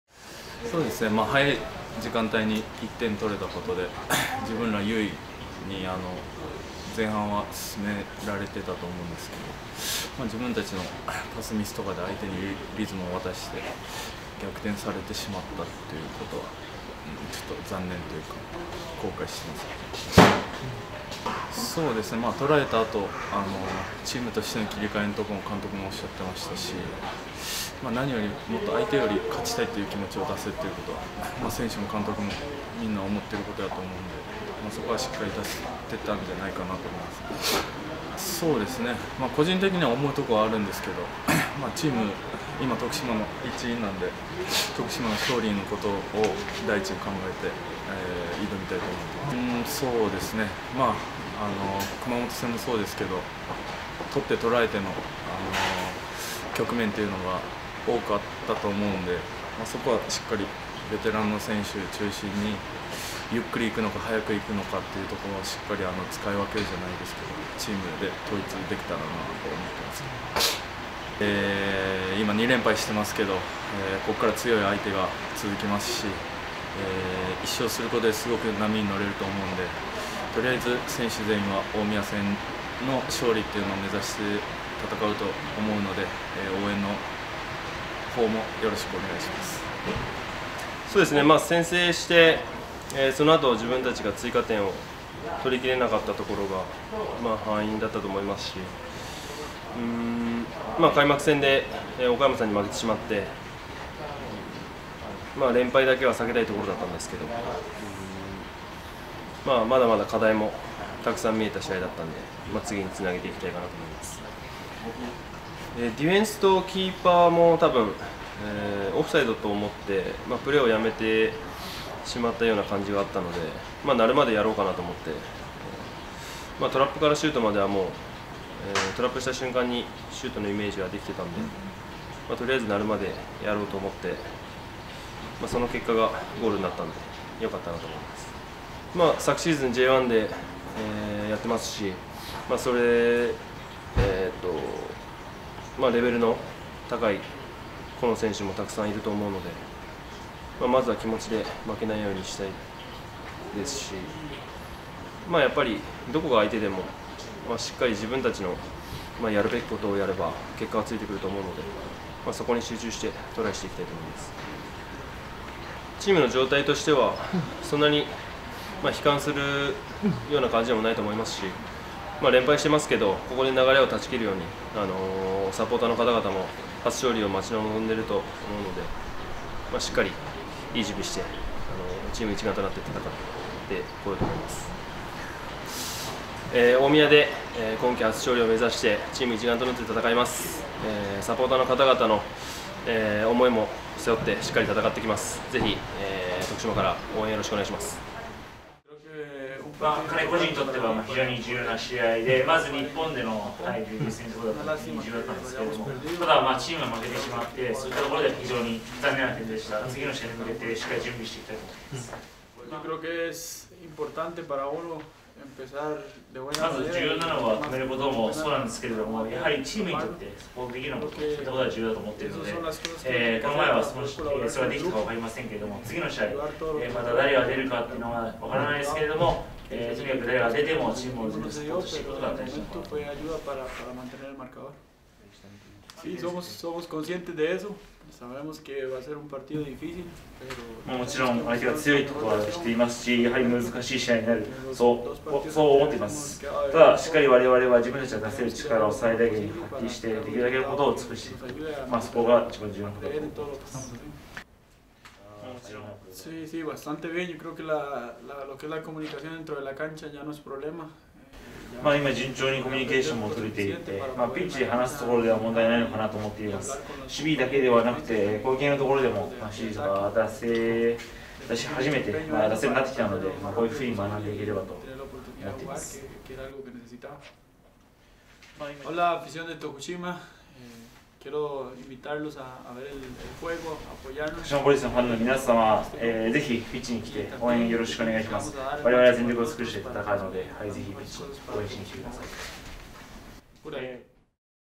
徳島ヴォルティス2018 J2第3節・大宮戦に臨む
大宮アルディージャ戦に向けて調整に励む徳島ヴォルティスのDF大屋翼選手、MF島屋八徳選手、GKカルバハル選手に意気込みとサポーターへのメッセージを伺いました。